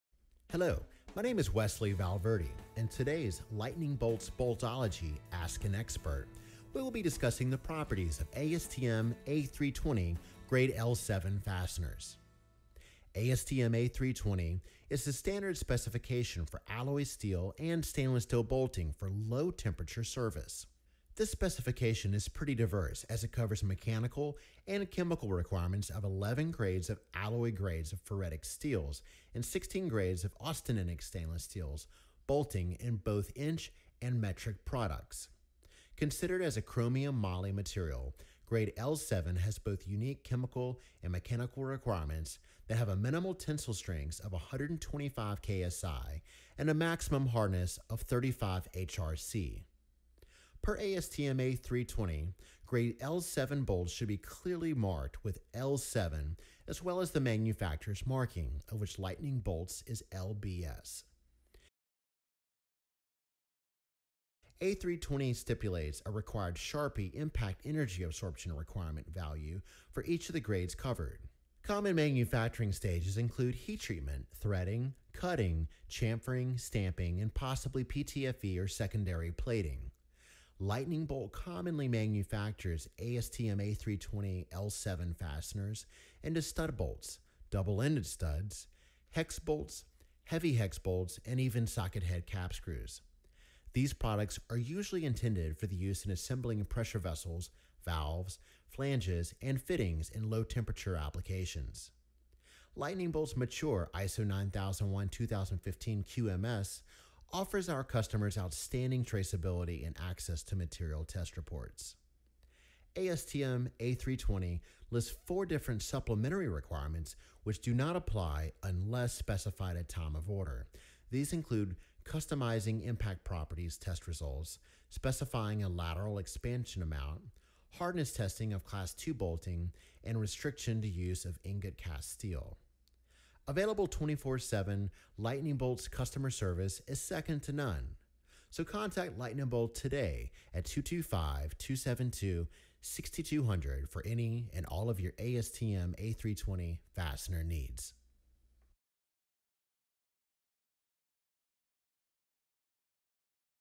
ASTM A320 L7 STUD BOLTS
A320 L7 studs are the most common type of product made of this grade, but , A320 L7 grade fasteners can be made into L7 hex bolts, L7 socket head cap screws, and L7 double ended studs are certified mechanically and chemically by their corresponding MTRs.  ASTM A320 grade L7 is an alloy that has intrinsic and unique properties that make applicable for use in low temperature service conditions.  to learn more.